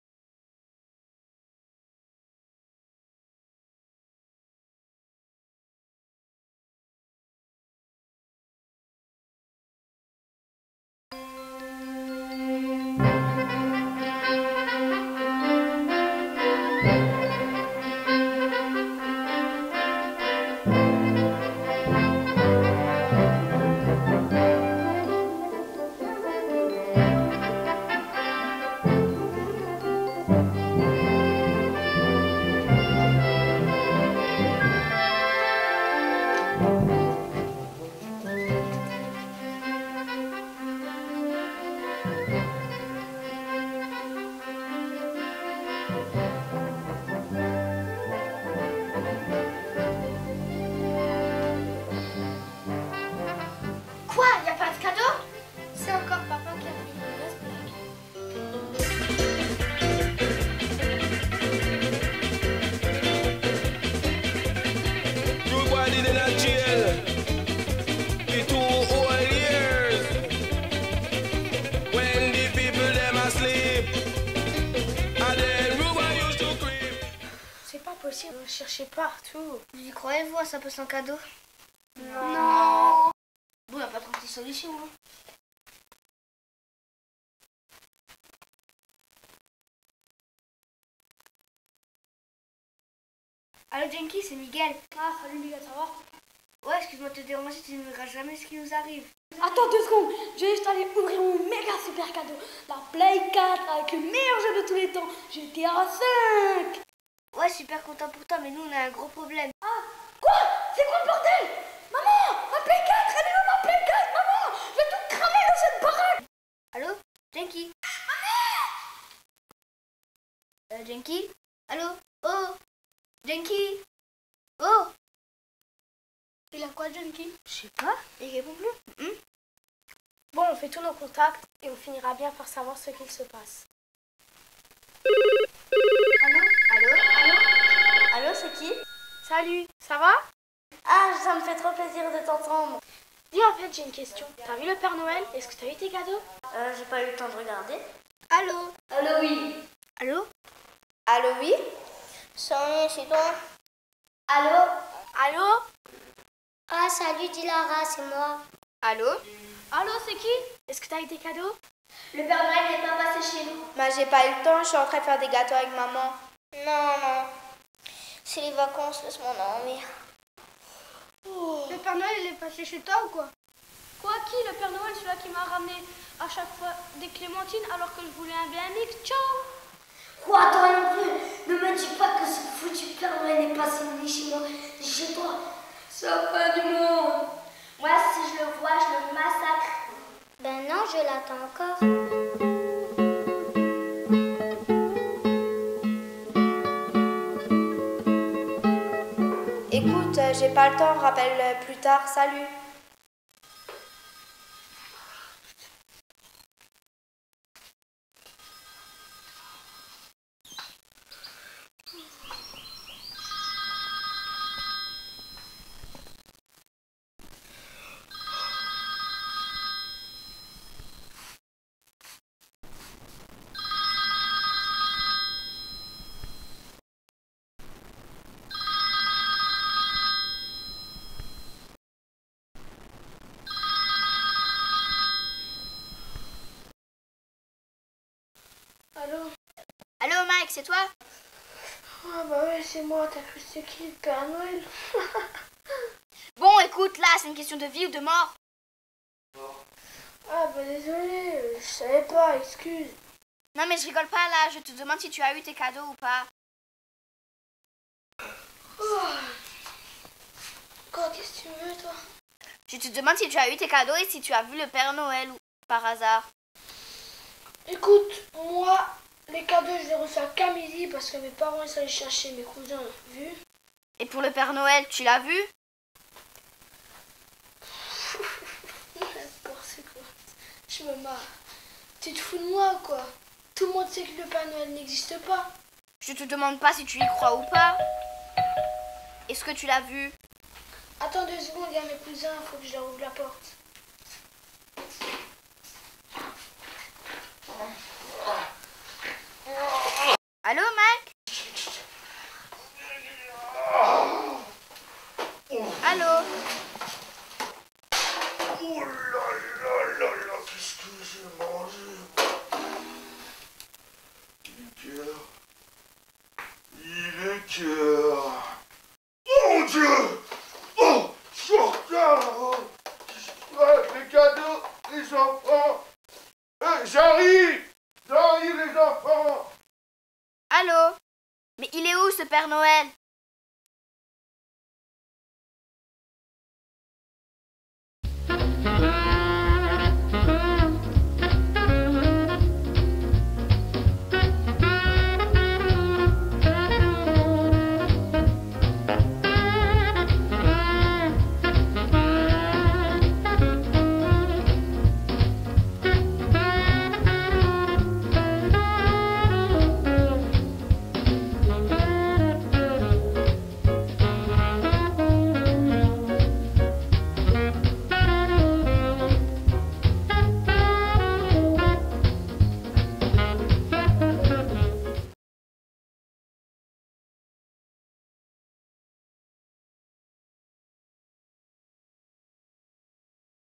la surprise de noel 2014
court métrage  des jeunes de la Maison de Quartier des Pins à Haguenau...réalisé en 2 semaines seulement
joyeux noël à tous et bonne année!!!